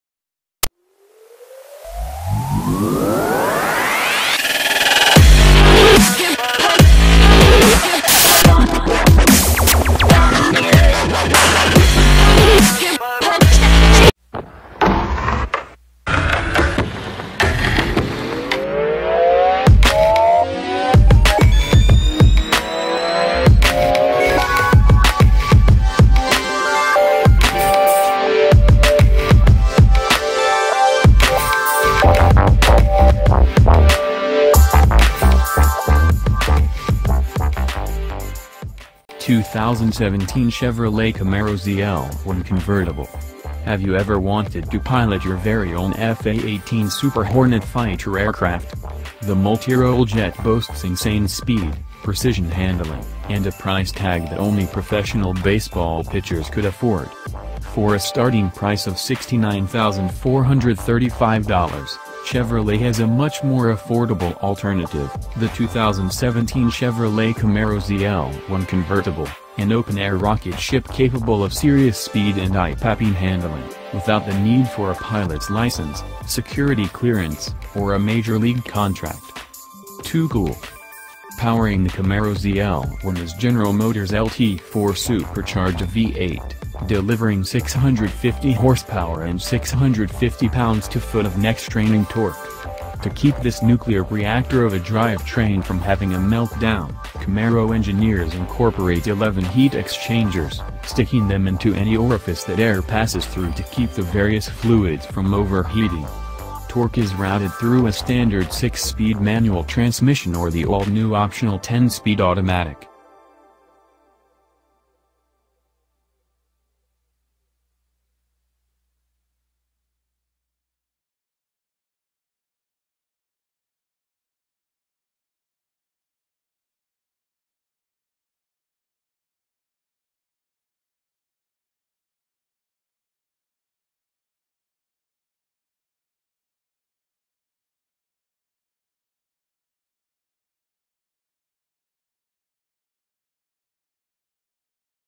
2017 Chevrolet Camaro ZL1 Convertible
Have you ever wanted to pilot your very own F/A-18 Super Hornet fighter aircraft? The multirole jet boasts insane speed, precision handling, and a price tag that only professional baseball pitchers could afford. For a starting price of $69,435, Chevrolet has a much more affordable alternative: the 2017 Chevrolet Camaro ZL1 convertible, an open-air rocket ship capable of serious speed and eye-popping handling, without the need for a pilot’s license, security clearance, or a Major League contract.